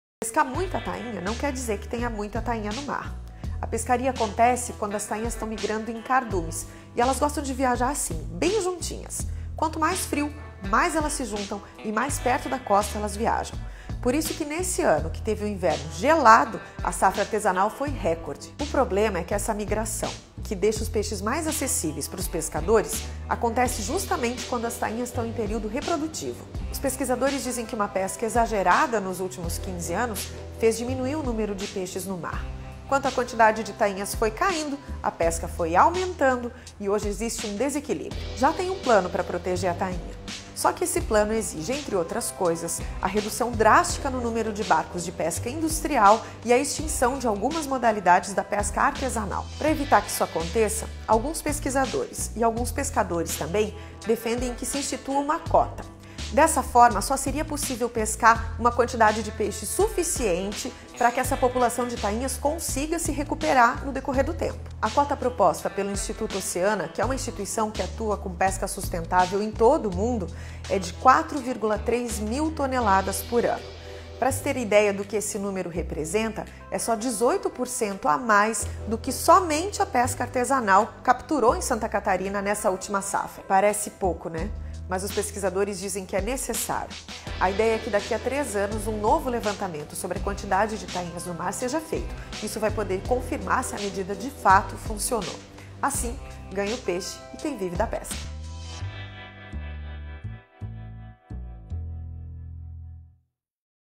A situação da pescaria da tainha nas regiões Sul e Sudeste do Brasil
Em vídeo produzido pelo Diário Catarinense, a jornalista Dagmara Spautz explica a atual situação da pescaria da tainha nas regiões Sul e Sudeste do Brasil. Mostra, também, a proposta apresentada pela Oceana para garantir uma pesca sustentável.